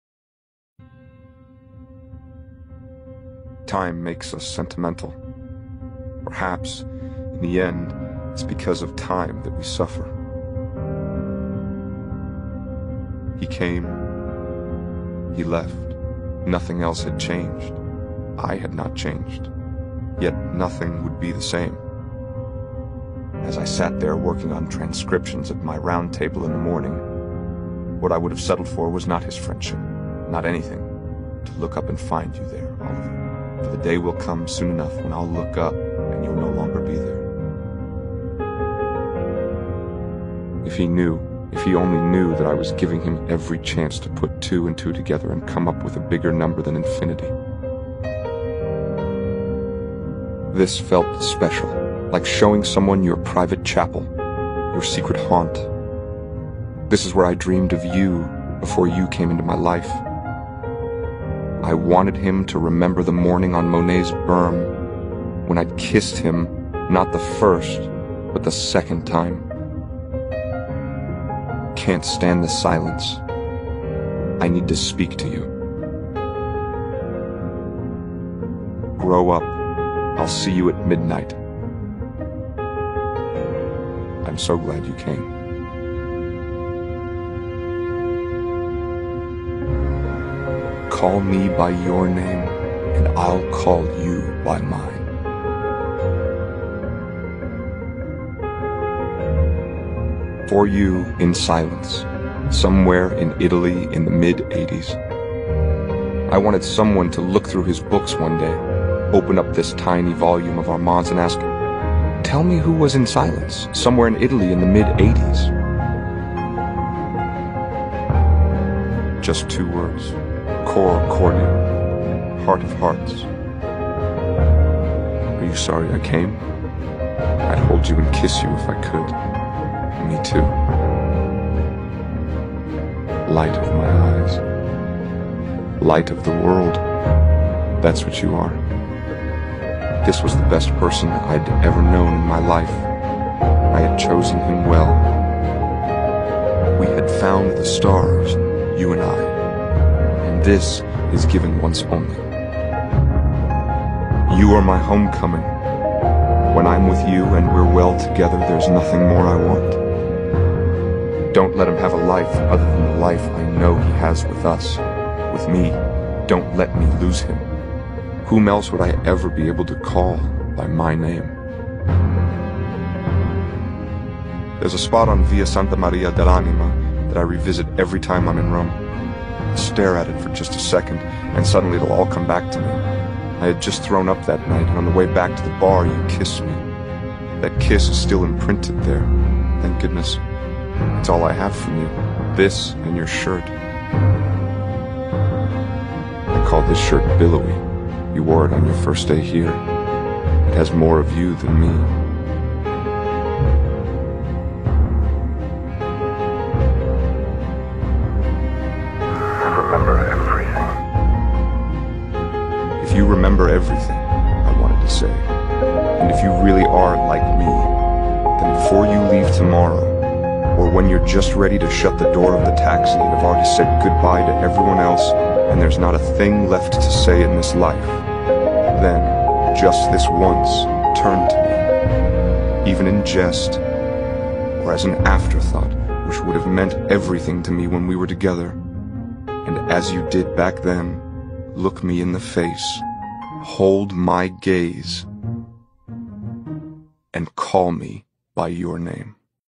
Call me by your name (Audio Book) | Read by Armie Hammer (Oliver)
This video was created and uploaded by Atlantis, the name is "Nothing will be the same" and you can find it on Youtube too.